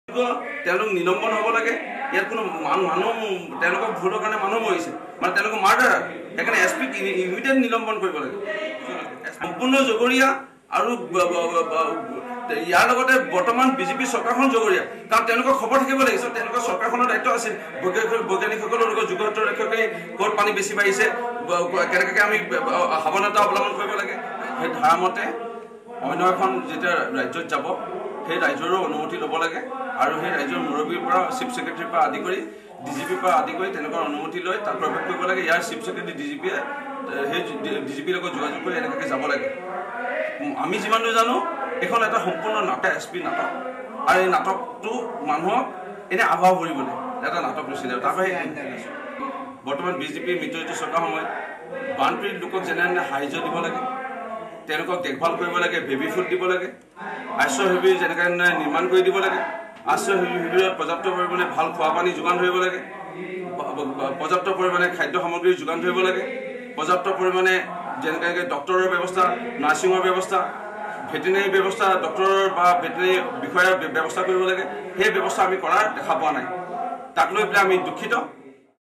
নীপকোৰ সঞ্চালকক গ্ৰেপ্তাৰ কৰা ঘটনাক বহুৱালি আখ্যা বিধায়ক ৰূপজ্যোতি কুৰ্মীৰ
গোলাঘাটৰ উপায়ুক্ত আৰু আৰক্ষী অধীক্ষকক হত্যাকাৰী আখ্যা বিধায়ক ৰূপজ্যোতি কুৰ্মীৰ । নীপকোৱে পানী মুকলি কৰি দিয়া বিষয়টোত গুৰুত্ব প্ৰদান নকৰাৰ বাবেই নিৰীহলোকে প্ৰাণ হেৰুৱাইছে বুলি অভিযোগ উত্থাপন কৰিছে বিৰোধীদলৰ বিধায়কজনে ।